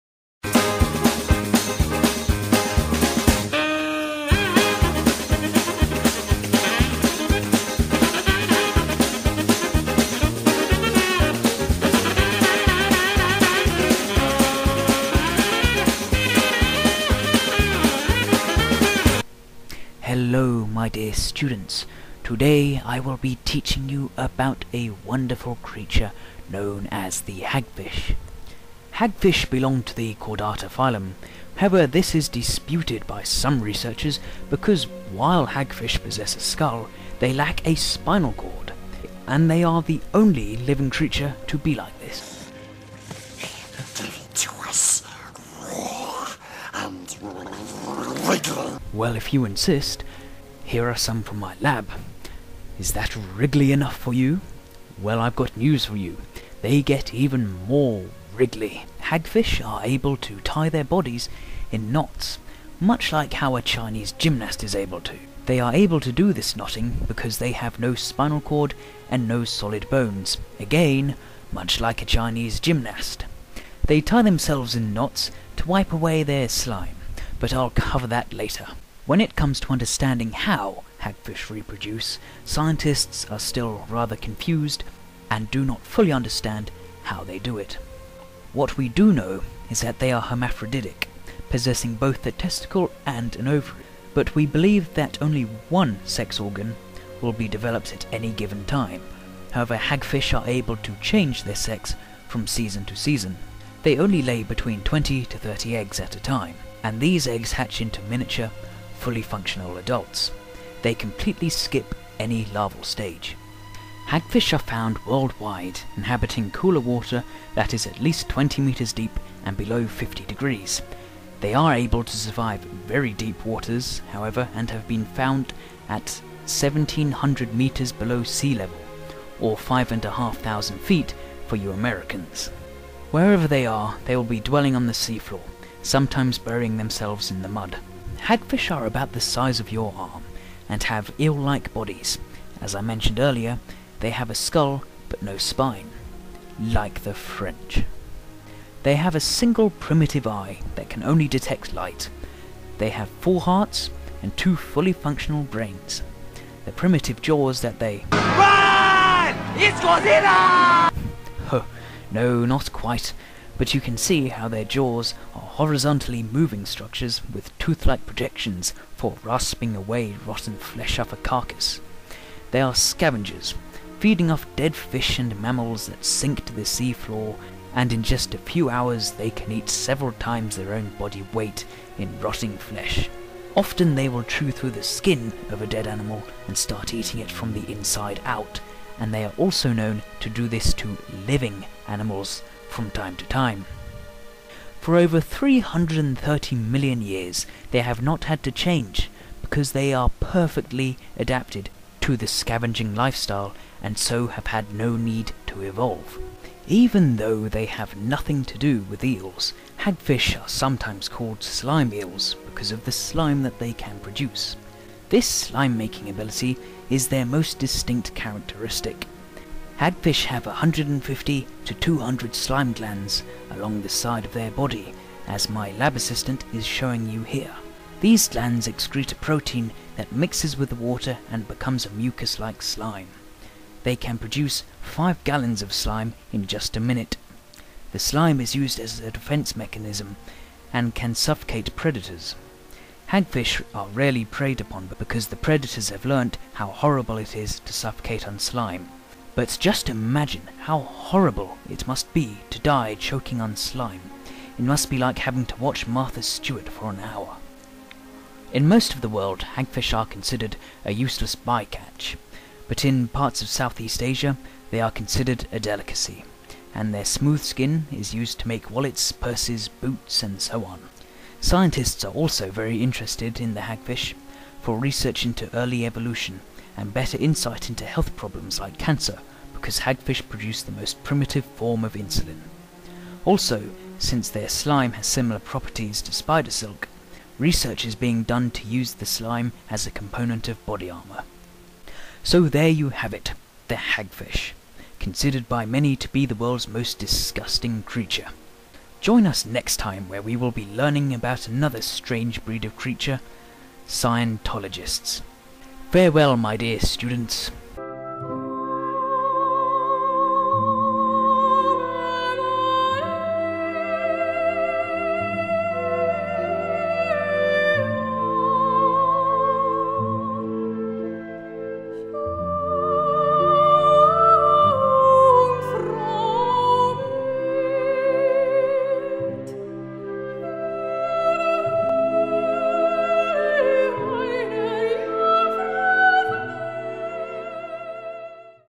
The Wacky and Wonderful World of the Hagfish
A science project I had to do for Marine Biology. We had to pick a marine animal and teach the class about it.